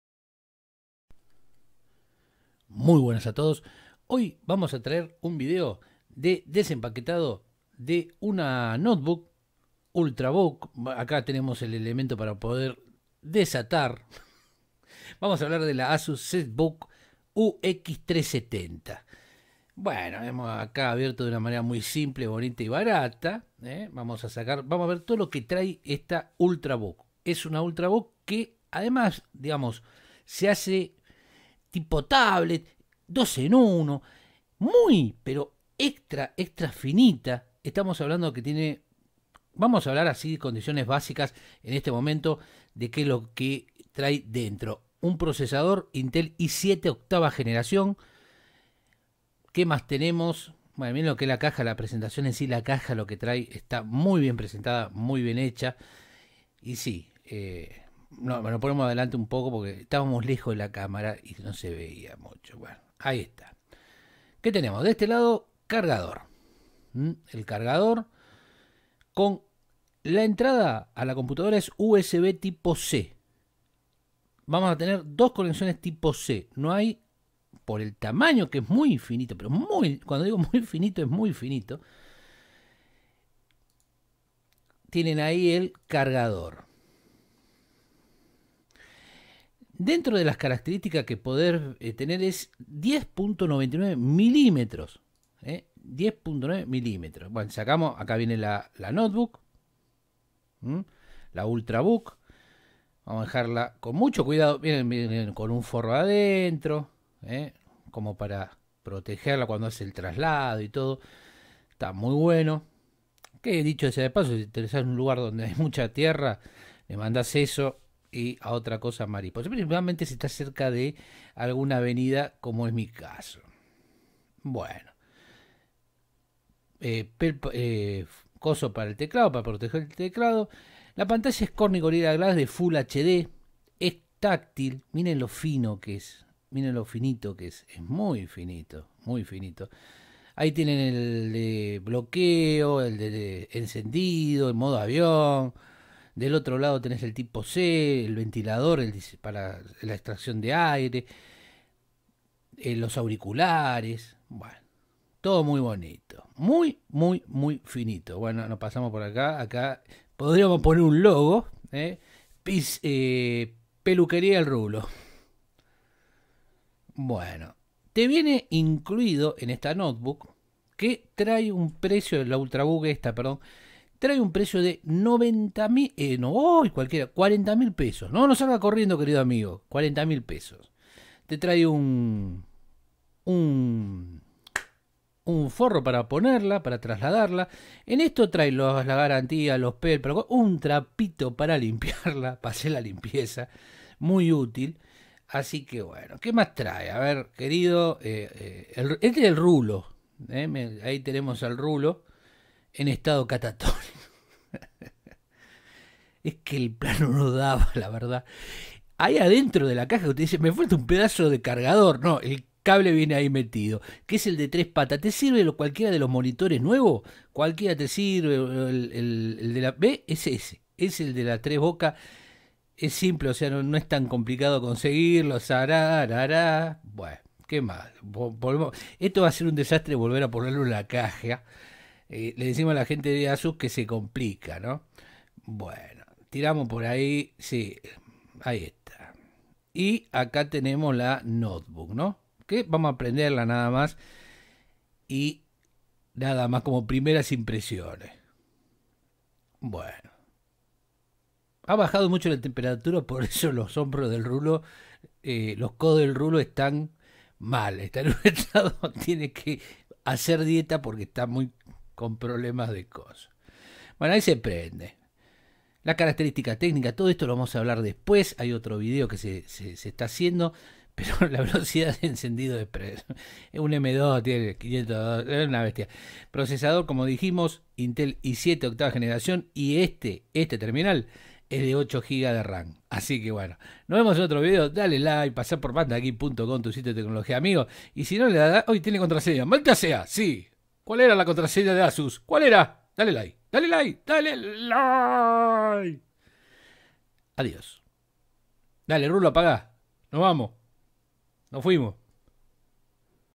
Unboxing Ultra Book Asus Zenbook UX370
Nos llego el desempaquetado de una Ultra Book Asus Zenbook UX370,